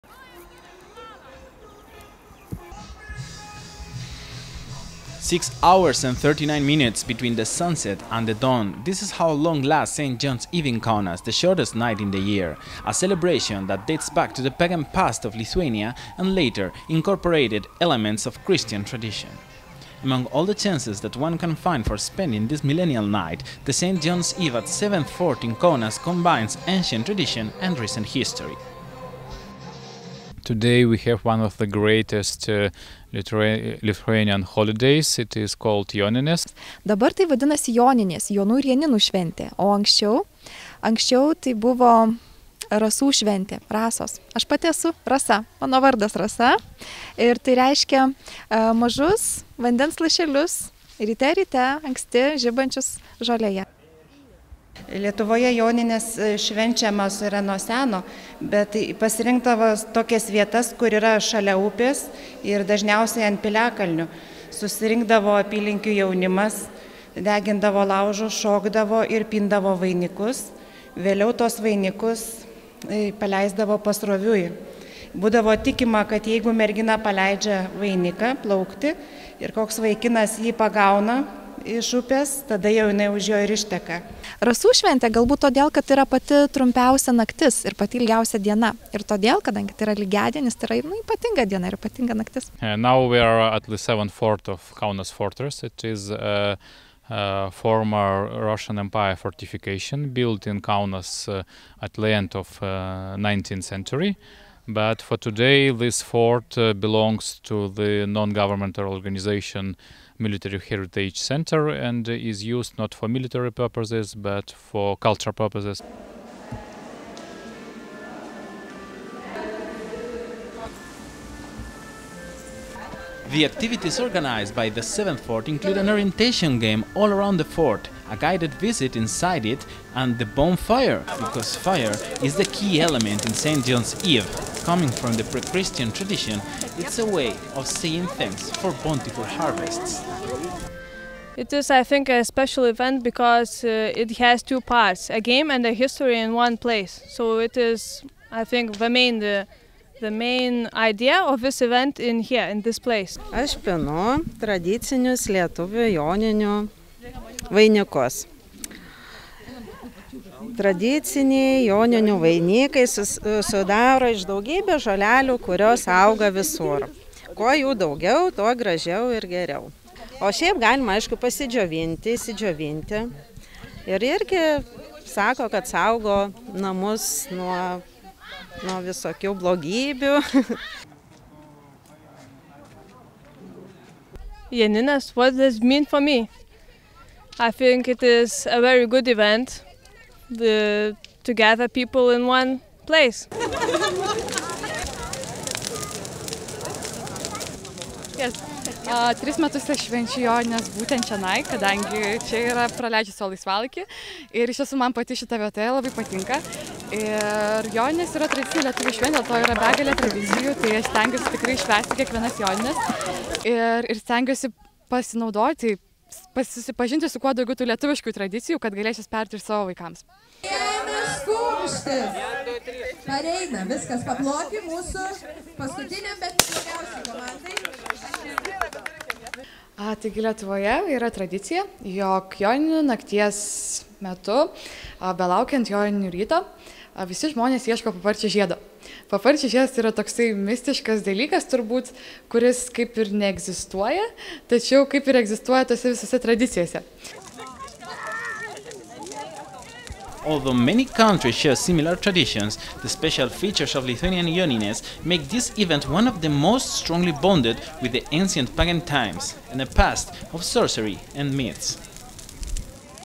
Saint John's Eve at Kaunas 7th Fort
Celebration of Saint John's Eve in the 7th Fort of Kaunas, Lithuania.